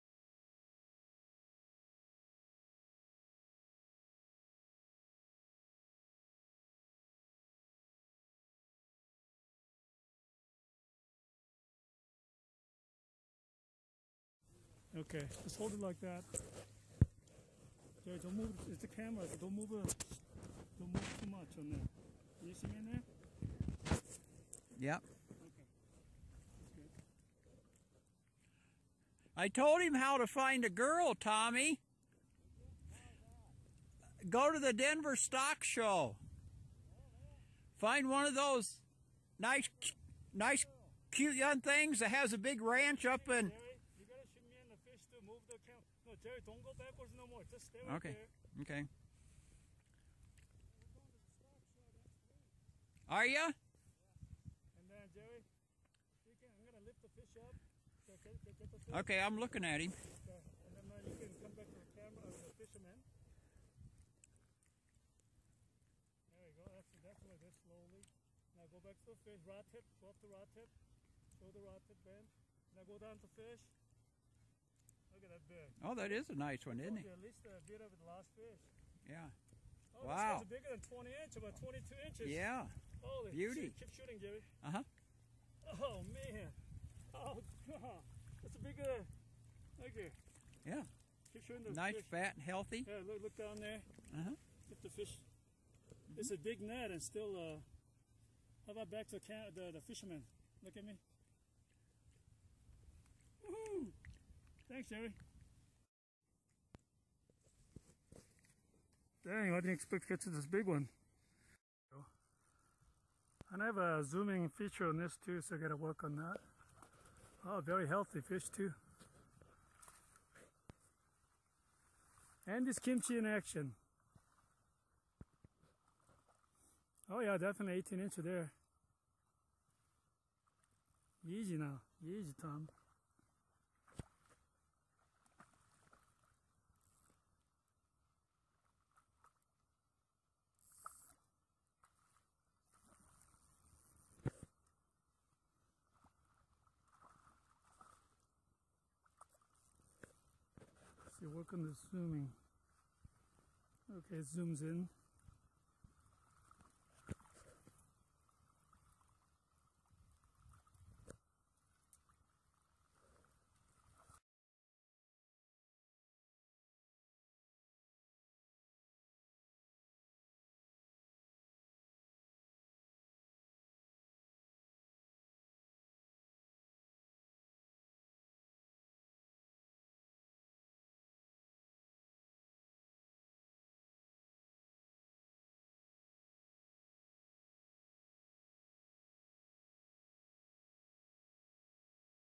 Big San Juan River trout Fly Fishing
Fished below the dam, not many people have been fishing there so trout were healthy and also hungry for my flies. Got waterproof case on my Samsung S4 so I can take it to anywhere without hesitation. This phone takes excellent photos and videos and I love it.